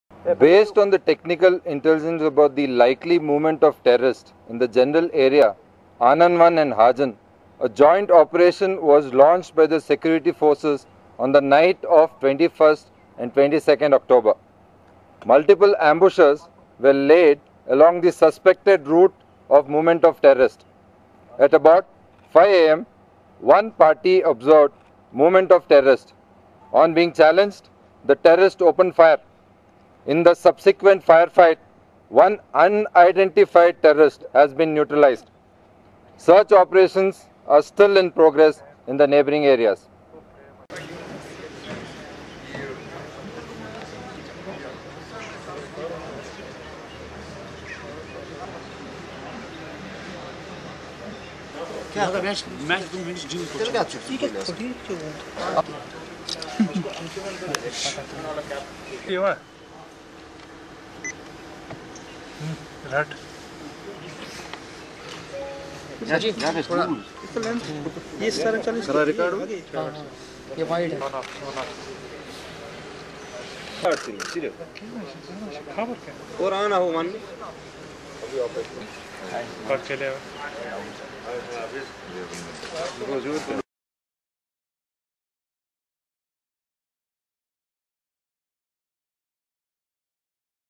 Handwara Ceasefire
Army's 9 Para a Nd SOG launched a cordon-and-search operation in Hajin area of Kralgund today at around 5:00 am